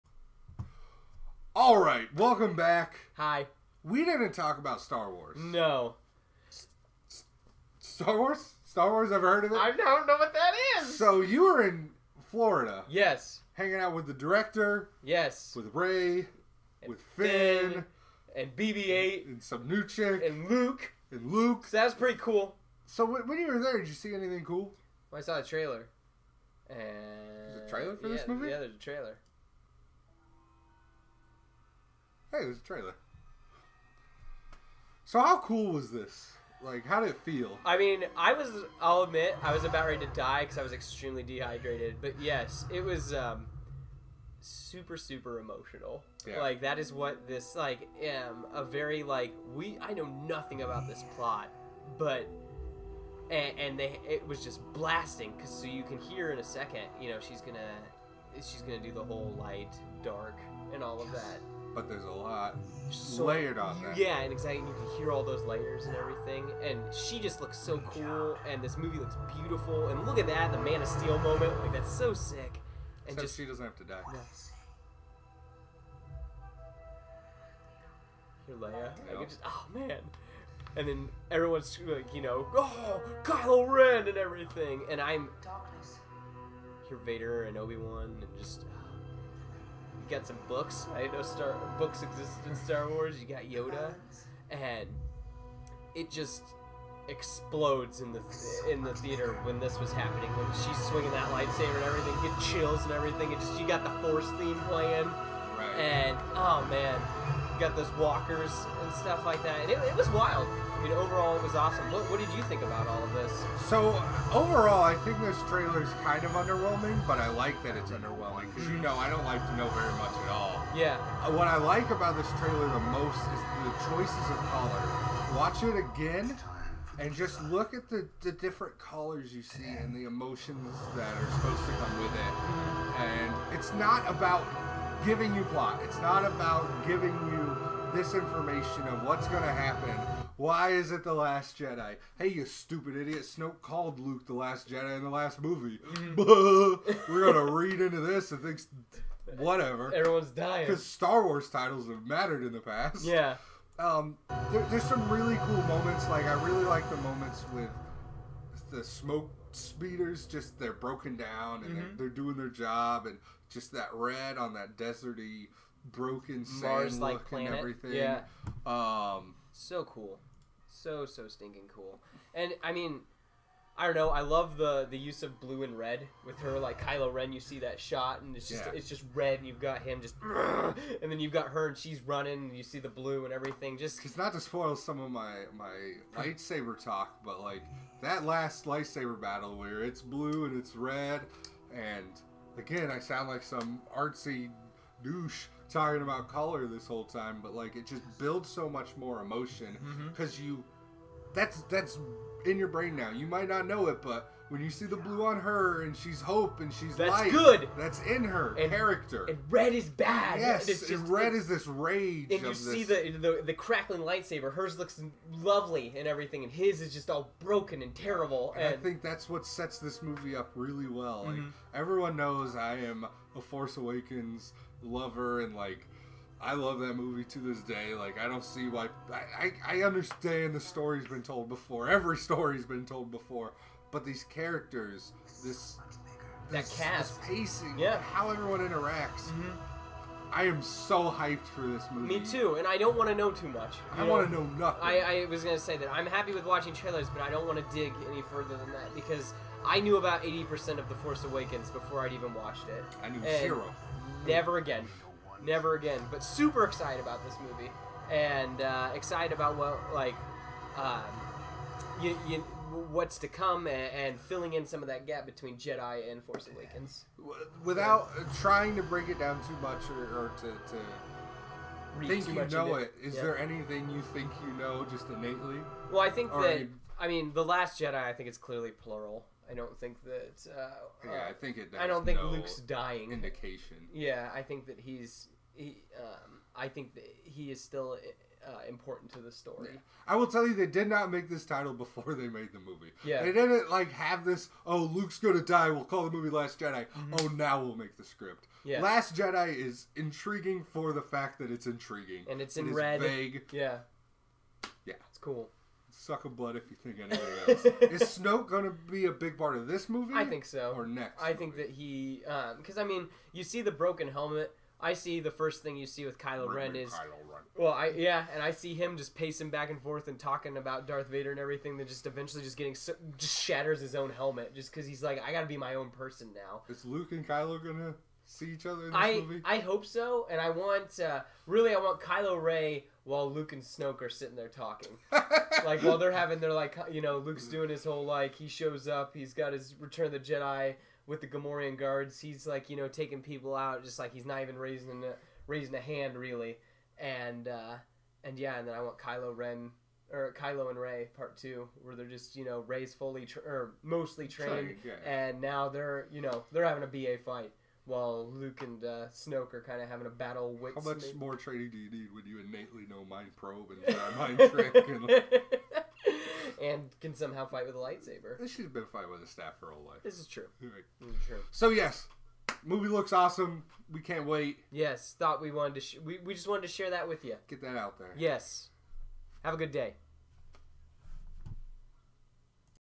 TNATG: The Last Jedi Trailer Talk
The geek was in Florida with the cast and crew as they premiered the tailer for Star Wars The Last Jedi.

We are late to the party but lets talk a little Star Wars.